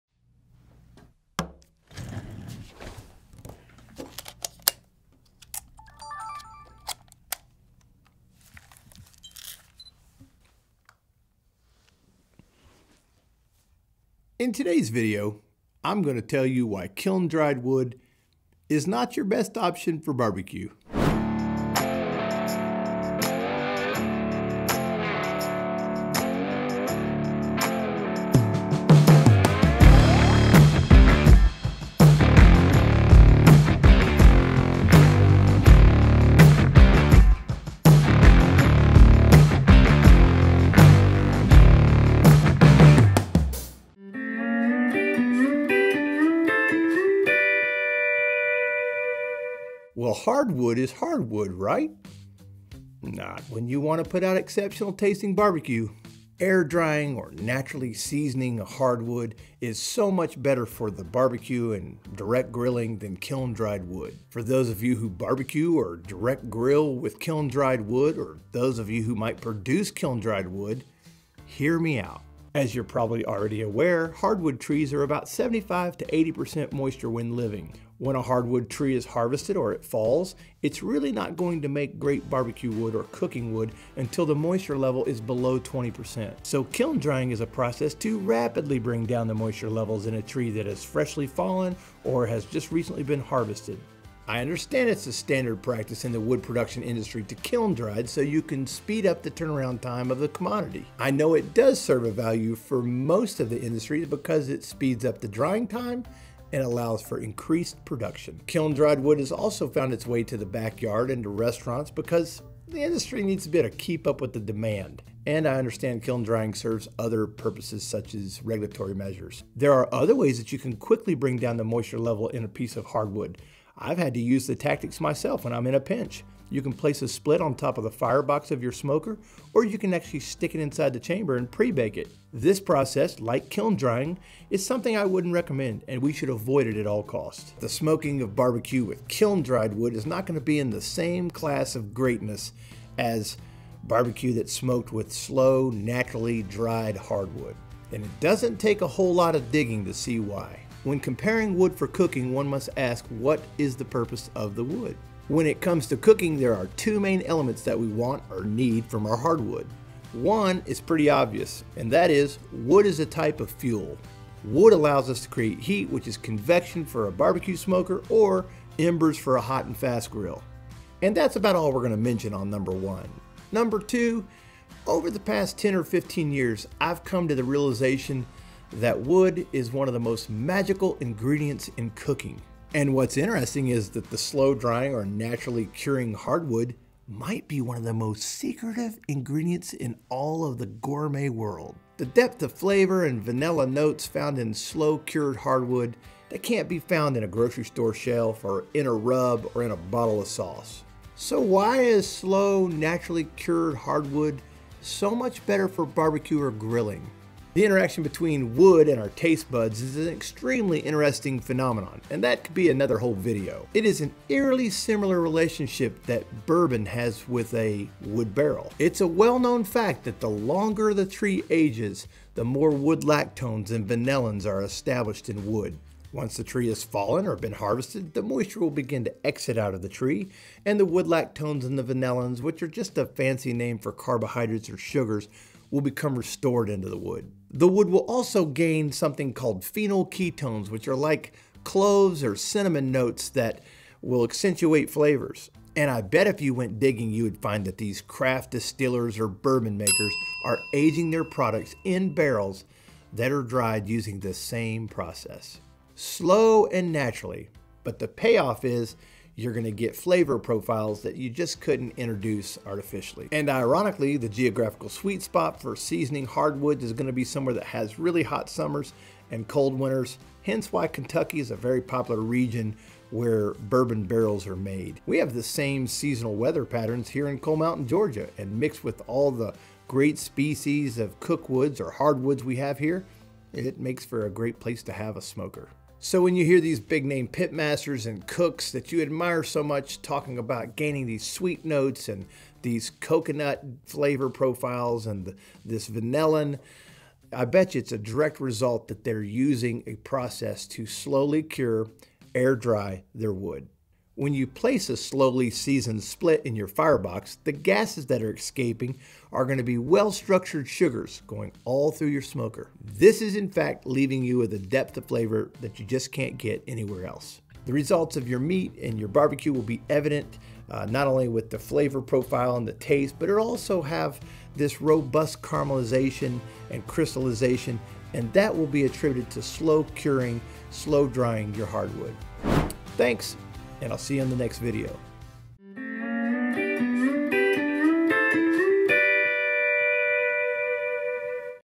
Cooking With Wood... Kiln Dried -vs- Naturally, Air Dried Hardwood
When smoking BBQ or cooking with wood, you can go a little or a long ways down the rabbit hole of excellent tasting food.  I am obsessed with ingredients and there is a huge difference between hardwood that is naturally seasoned, verses kiln dried (or fast dried wood).